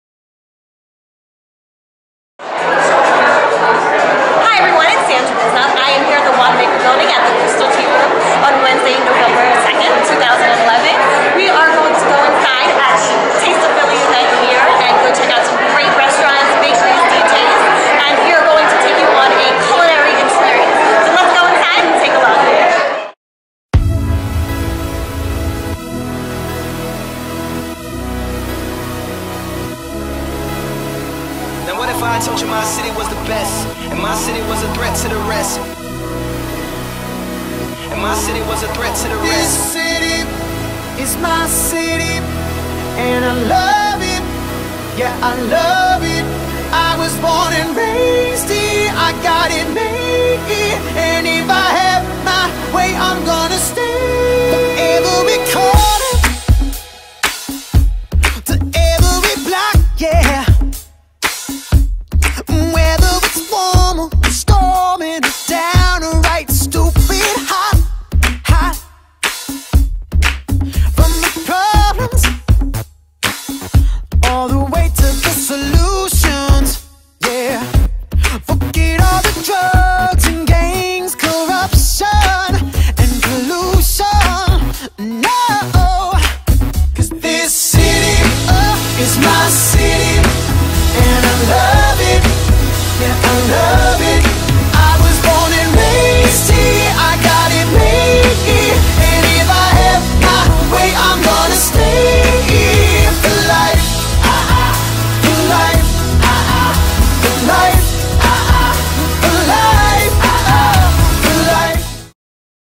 Taste of Philly 2011 with Sandra Visnov
Taste of Philly was an evening sampling some amazing food and drinks from the city's most delectable restaurants & top chefs.  A great atmosphere that created enjoyment for all five senses!  A portion of proceeds went to benefit City of Hope and Philabundance, bringing more healthy foods to area schools and neighborhoods.